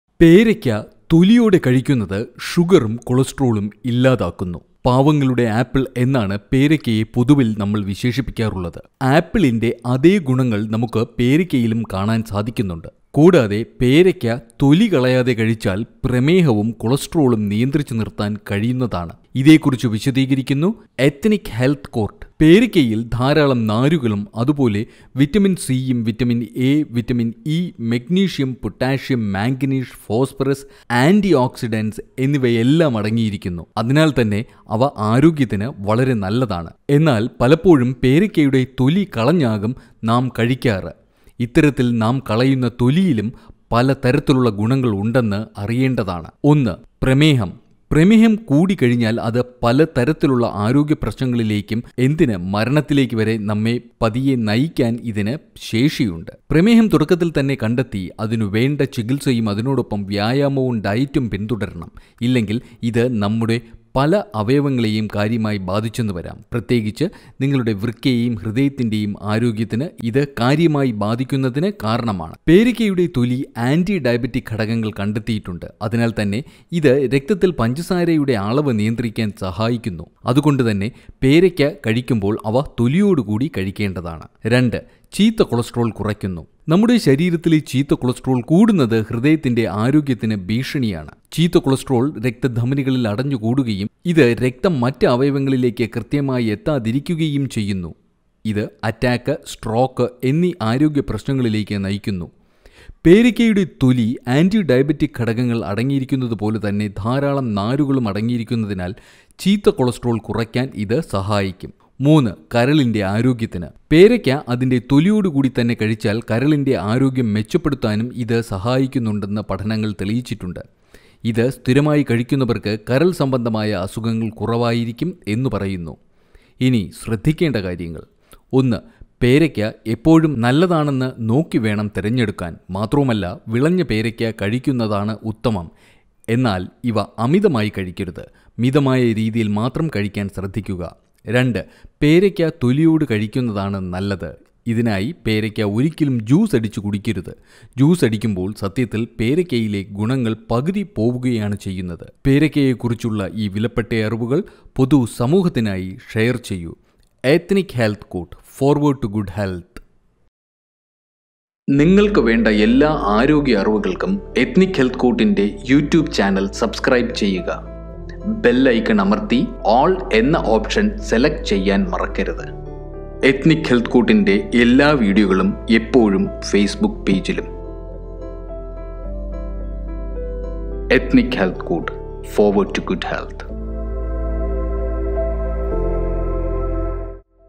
പേരയ്ക്ക തൊലിയോടു കൂടി കഴിച്ചാൽ പ്രമേഹം കൊളസ്ട്രോൾ നിയന്ത്രിക്കാം | Guava With Peel
നിറയെ ആരോഗ്യഗുണങ്ങൾ നിറഞ്ഞ ഒന്നാണ് പേരയ്ക്ക. ഇതില്‍ ധാരാളം നാരുകളും അതുപോലെ, വിറ്റമിന്‍ സി, വിറ്റമിന്‍ എ, വിറ്റമിന്‍ ഇ, മഗ്നീഷ്യം, പൊട്ടാസ്യം, മാഗ്നീസ്, ഫോസ്ഫറസ്, ആന്റിഓക്‌സിഡന്റ്‌സ് എന്നിവയെല്ലാം അടങ്ങിയിരിക്കുന്നു. ഇവ രോഗങ്ങളില്‍ നിന്നും നമ്മളെ സംരക്ഷിക്കുന്നതിനും സഹായിക്കുന്നു. അതുപോലെ, ഈ പേരയ്ക്ക തൊലി കളയാതെ കഴിച്ചാല്‍ പ്രമേഹവും കൊളസ്‌ട്രോളും എങ്ങിനെ നിയന്ത്രിക്കാന്‍ സാധിക്കും .